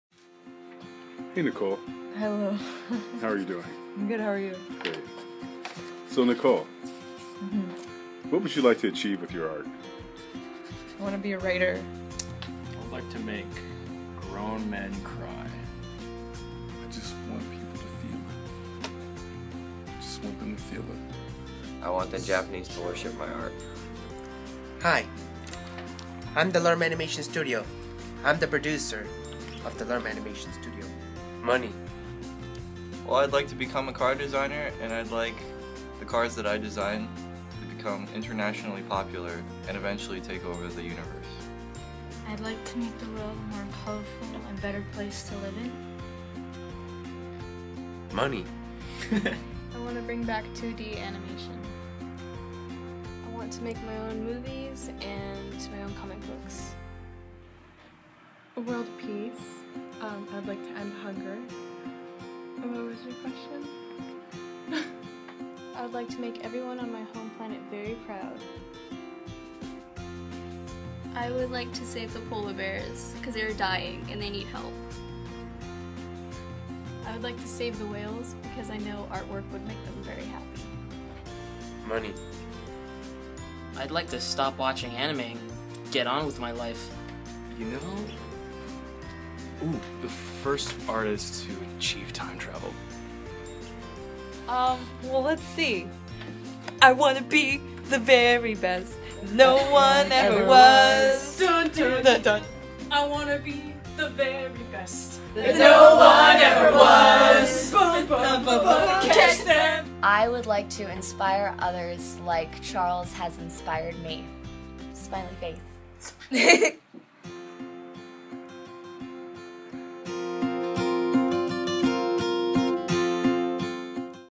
The Animation Academy - Aspirations with your art
Our students are as unique as they are talented with a wide range of career goals. In this video they share their aspirations for what they would like to achieve with their art.